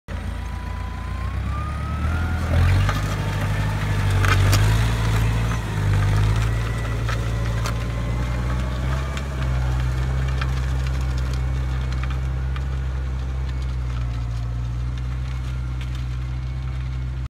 Last work in hop field
Plowing soil away and deep soil cultivation as the last work in the field before winter.